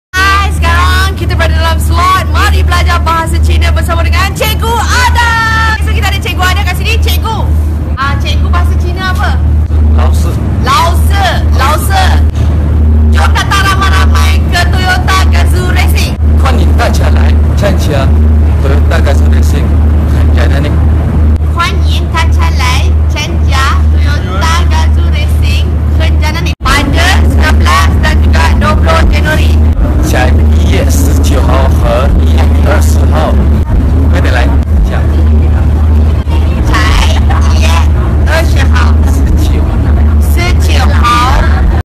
Janna Nick belajar bahasa Cina dengan Adam Lee
P/s @adm.lee tu cousin Janna Nick